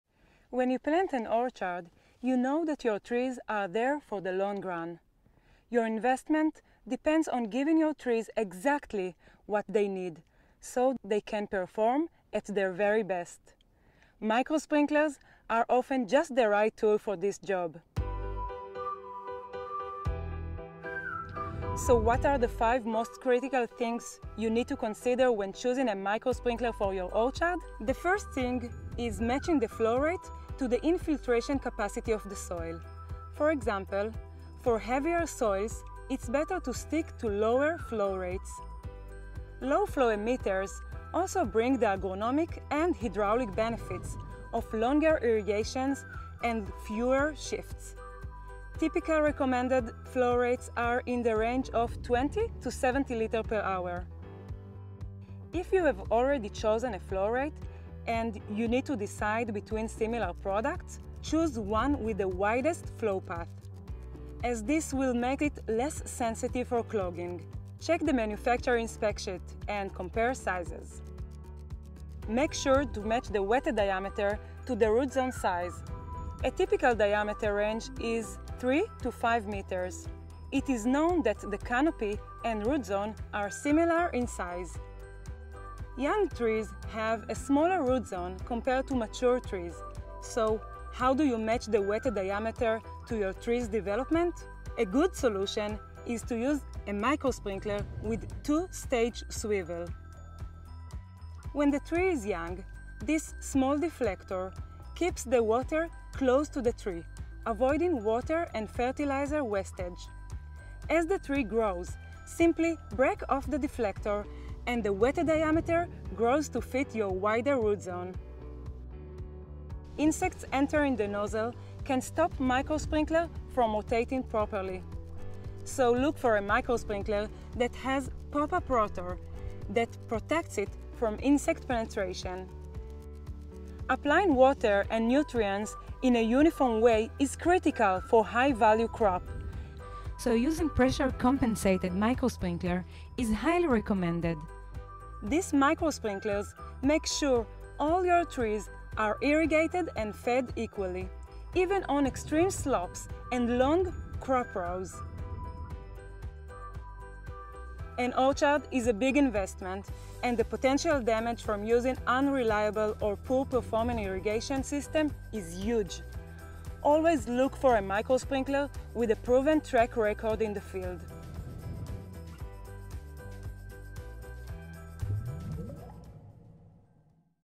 Micro sprinklers irrigation for orchard
See how Netafim's micro-sprinklers can help you get the maximum out of your orchards. Click here to find the sprinkler irrigation solution that works for you